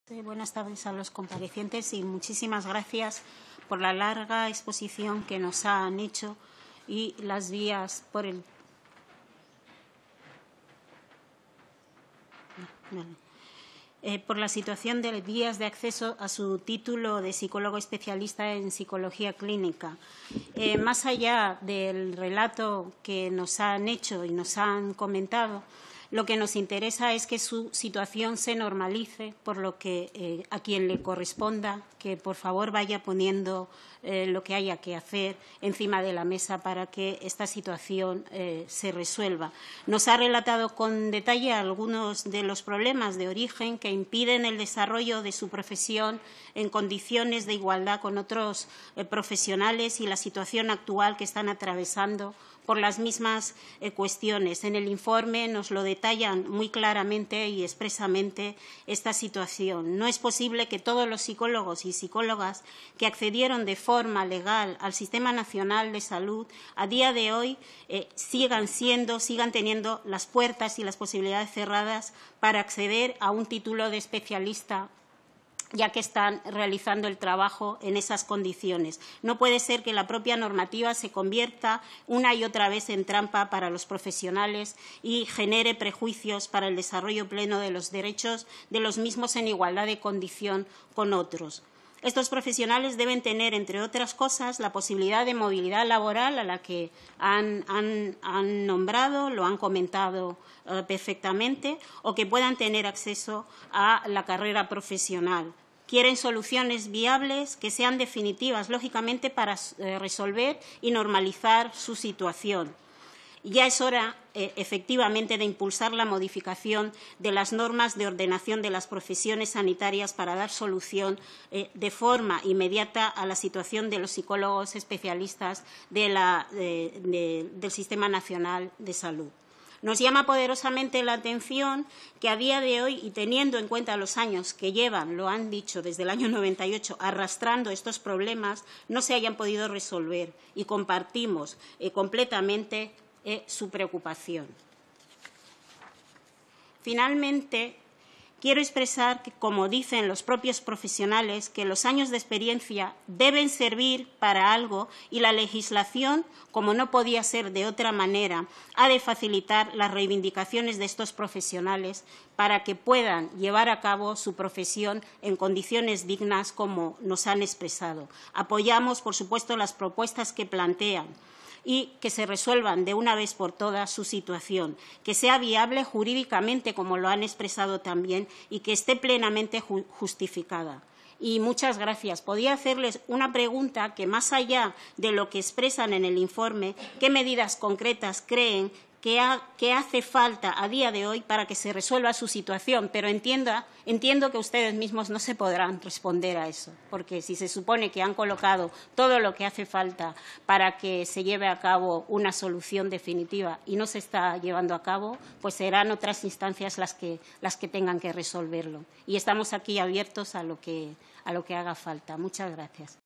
Rita Bosaho en la comisión de Sanidad y Servicios Sociales el 21 de marzo
Tras la comparecencia de doña Gemma Escuder, Presidenta de la Asociación de Psicólogos Especialistas del Sistema Nacional de Salud sin Titulación Oficial .